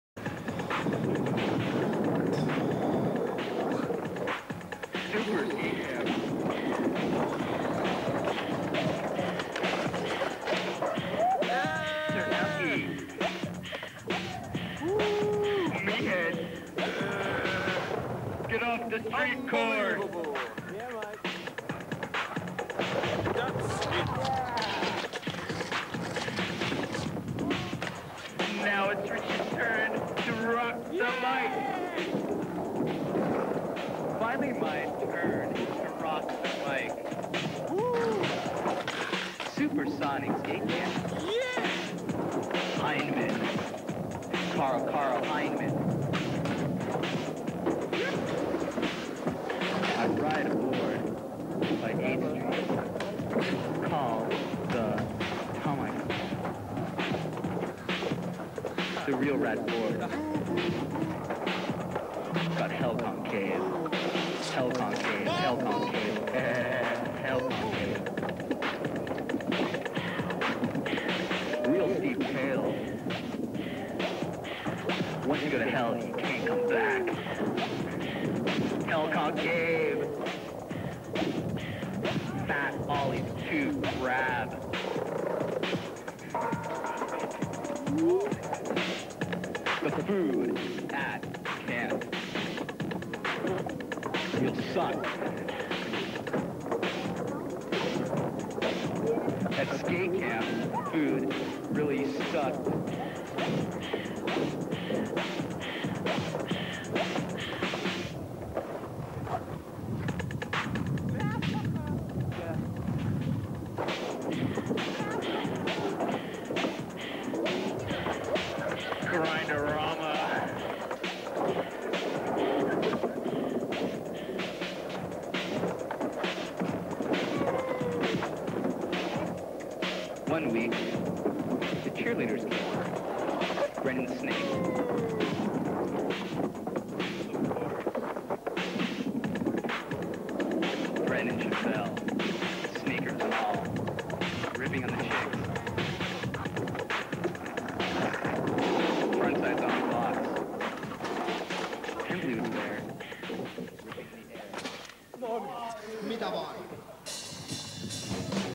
Tuukka Temosen skeitticlippi vuodelta 1999
Feat Sami Yli-Pihlaja. Kuvattu Heinolassa. En omista mitään